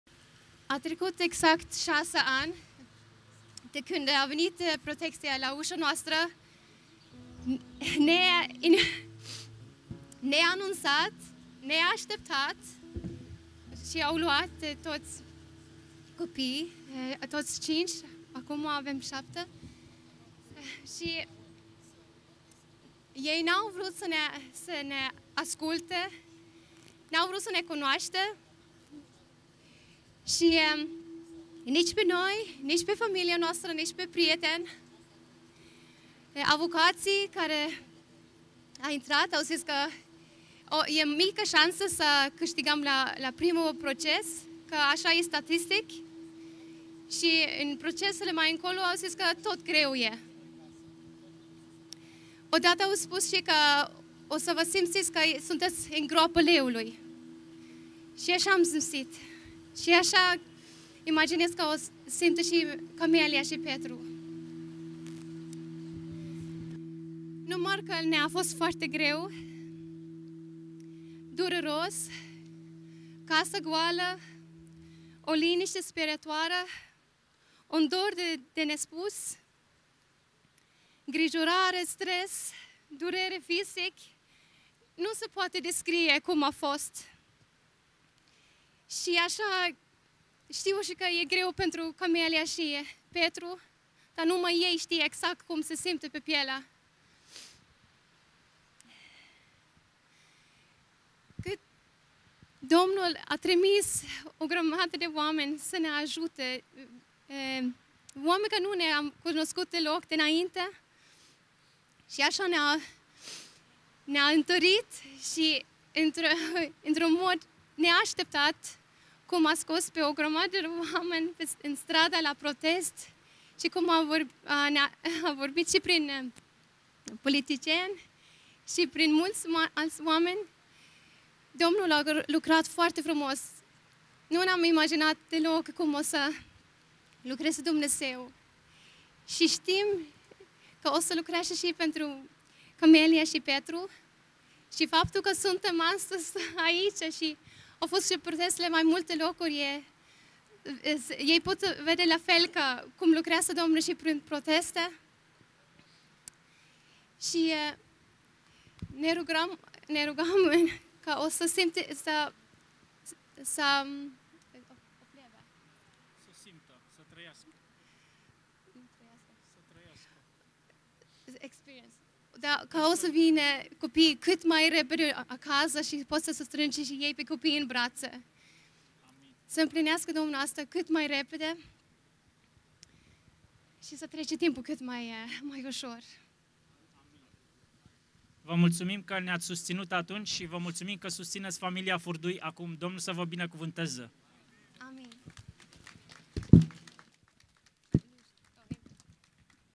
Ruth Bodnariu | Protest pentru susținerea familiei Furdui | București | 4 Decembrie 2021
CIF: 33686860